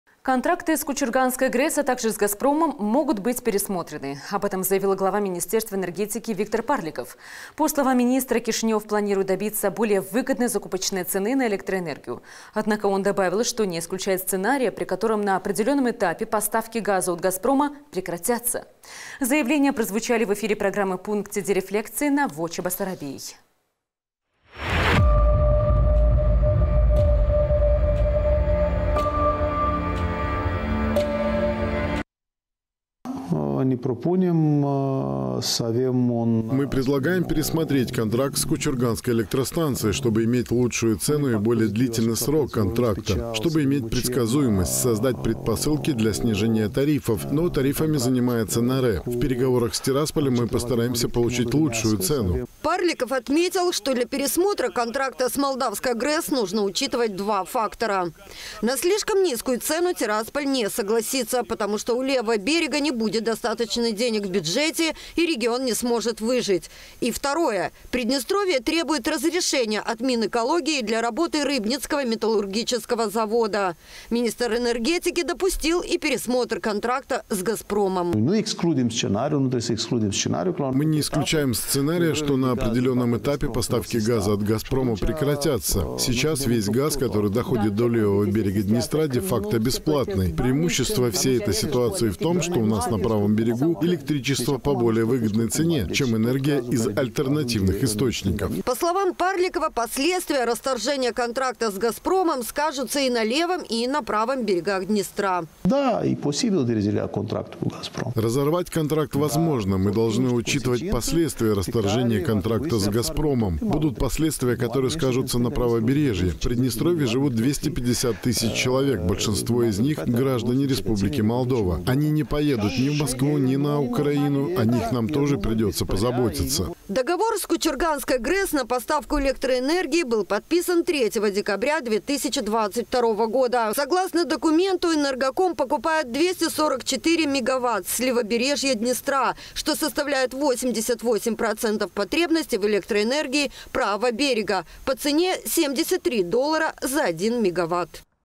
Первые шаги минэнэрго
Контракты с Кучурганской ГРЭС, а также с «Газпромом» могут быть пересмотрены. Об этом заявил глава министерства энергетики Виктор Парликов, на вопрос о первых шагах на посту министра. По словам Парликова, Кишинёв планирует добиться более выгодной закупочной цены на электроэнергию, но не питает иллюзий по поводу очень низкой цены, ибо от неё зависит существование приднестровского региона. Заявления прозвучали в эфире программы «Puncte de reflectie» на Vocea Basarabiei.